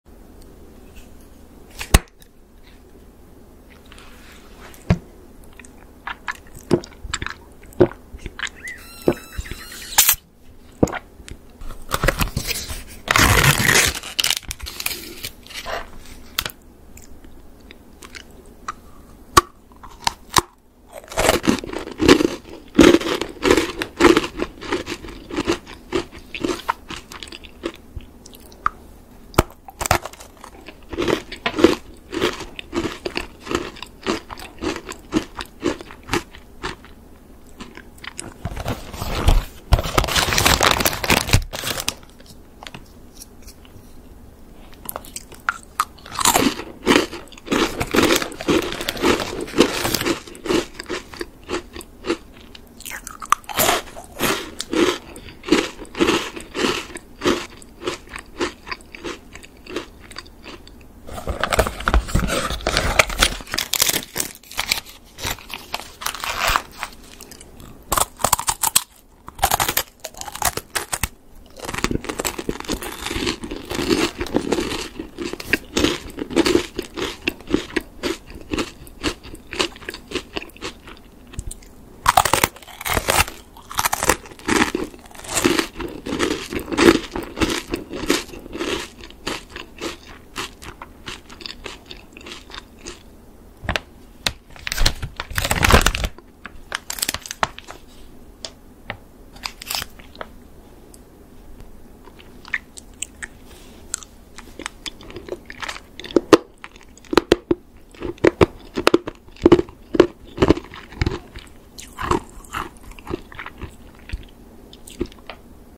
ASMR PORORO FOOD - PORORO CHOCO STICK, PORORO LYCHEE, PORORO ICE CONE, PORORO VITAMIN
Aku ASMR makanan Pororo sesuai emoji, Ada Pororo Choco Stick, Pororo Lychee, Pororo Ice Cone Chocolate, Pororo Chewable Vitamin C dan Pororo Crackers. Makanan Pororo favoritmu yg mana?